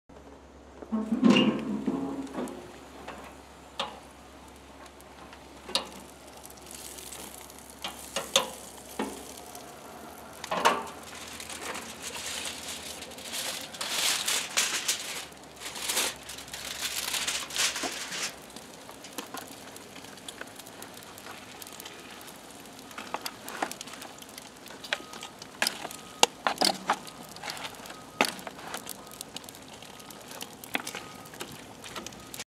Spicy Newf Getting Stirred Up!
The Spicy Newf pulled pork in its final stage heating up on the grill. It's cold here today, barely above freezing. It even snowed this morning...
So why not barbecue?!?!?